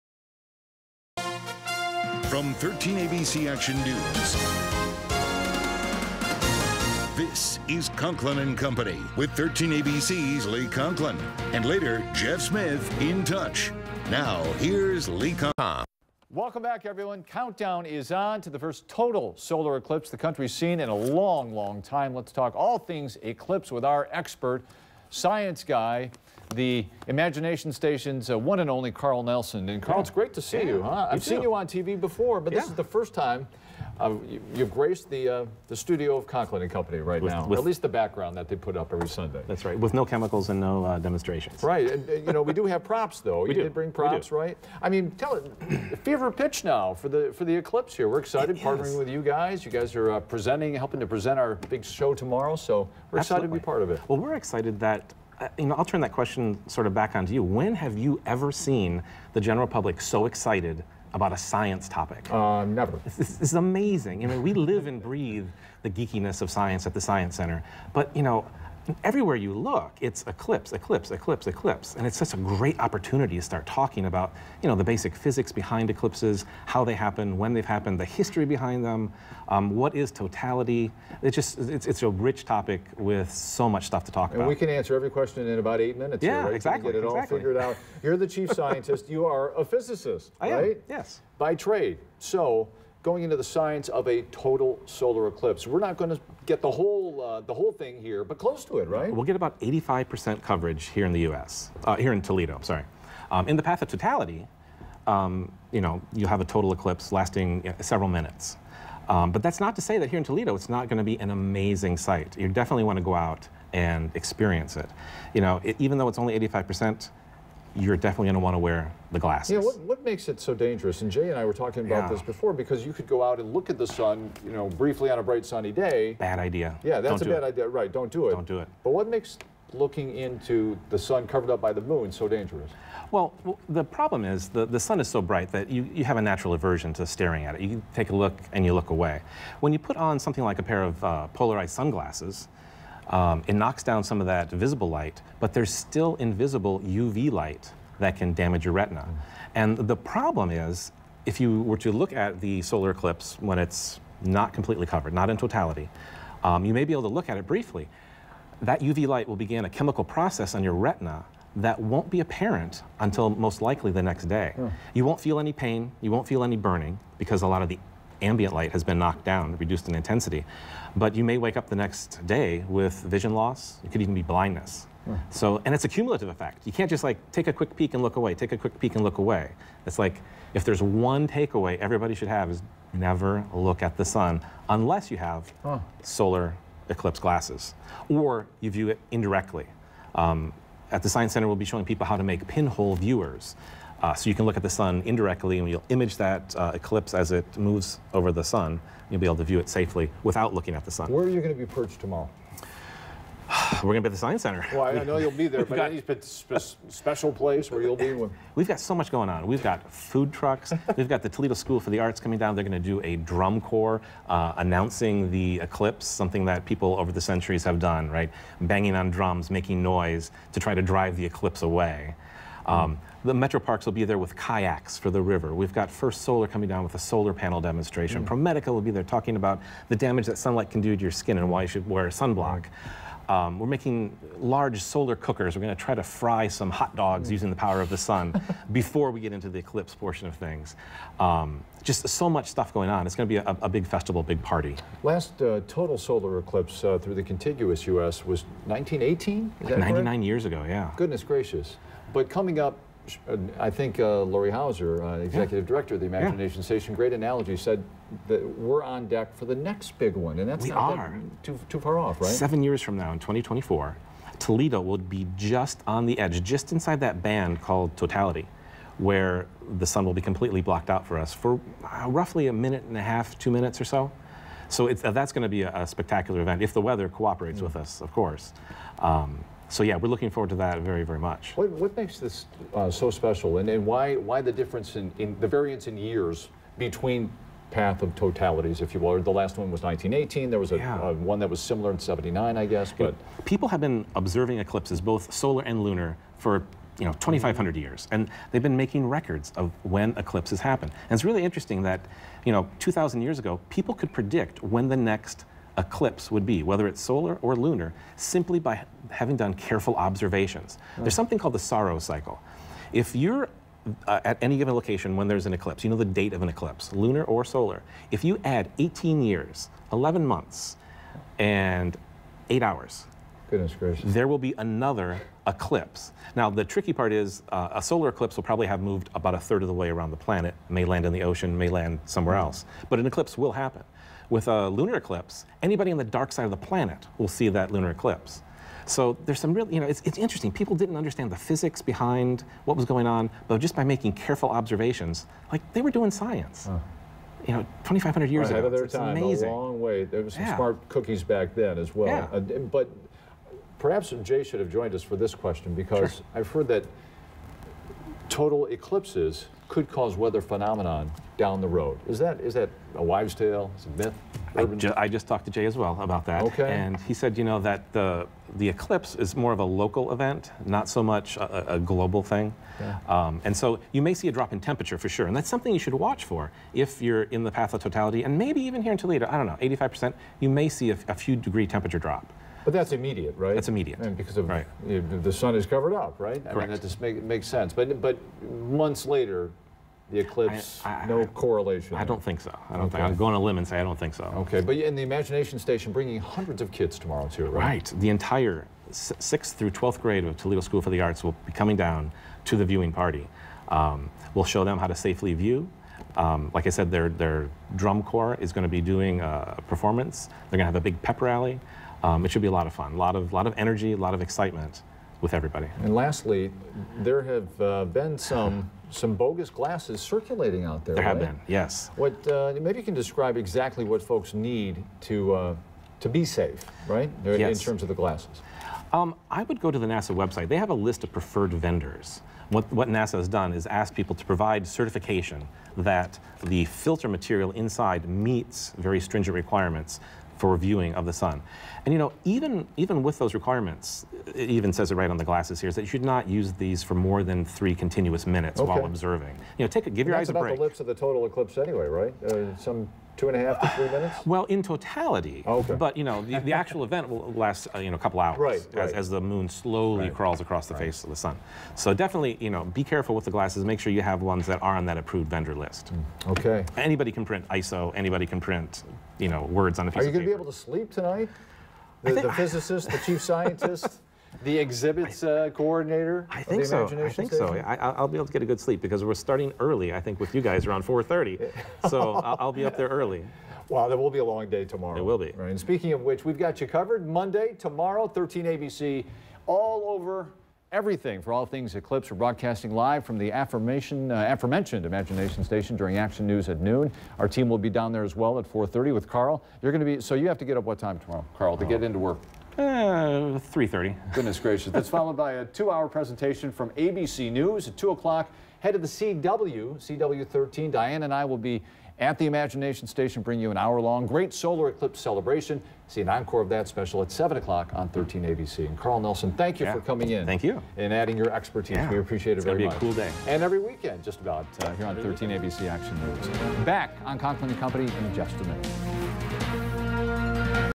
Conklin and Company interview about eclipse
Lee Conklin of 13abc interviews Chief Scientist Carl Nelson from Imagination Station in Toledo Ohio about the upcoming solar eclipse.